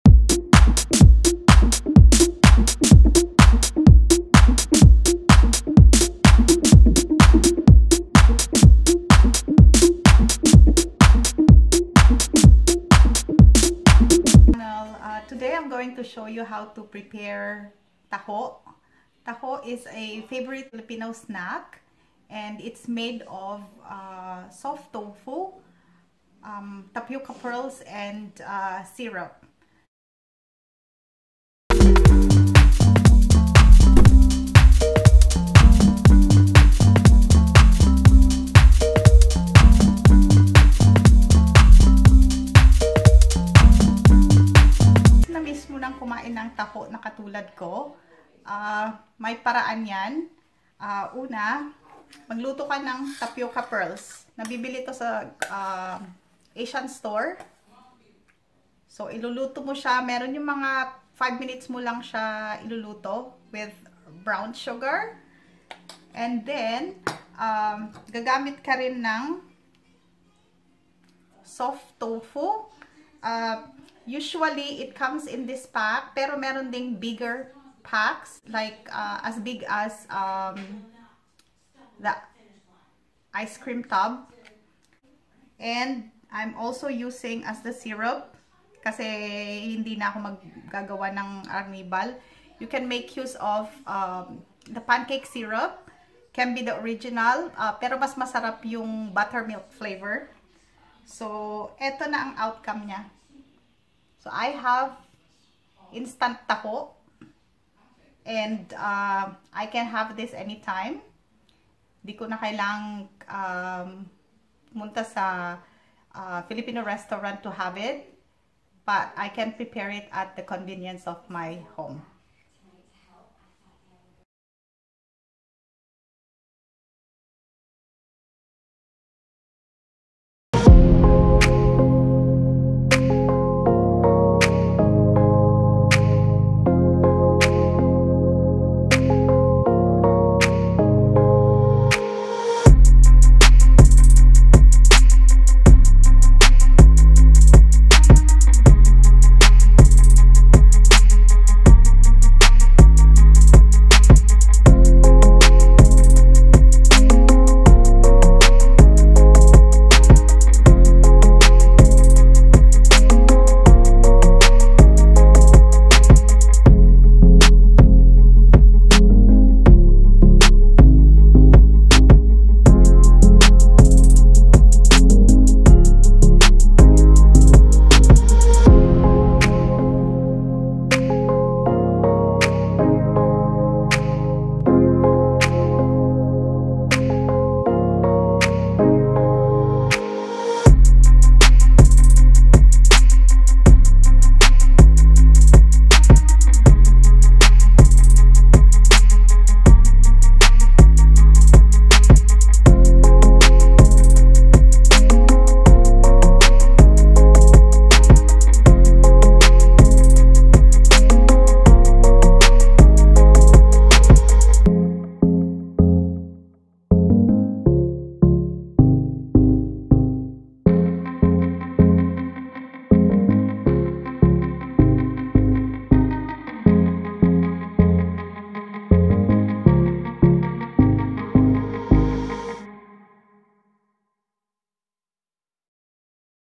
HOW TO PREPARE TAHO - EASY RECIPE | Filipino Life in Canada
HOMEMADE TAHO

Hi Everyone! I would like to share with you how I prepare Taho at home. Taho is a favorite Filipino snack made with soya beans, tapioca pearls and a special syrup made from sugar cane called 'arnibal'.

Making taho from scratch is a bit challenging for me, so I never attempted to make one.

But you can buy soft tofu and tapioca pearls from the Asian Stores  and use pancake syrup (original or butter flavor) to substitute the  arnibal. Try other syrups like maple or strawberry  too, they are equally good. Enjoy!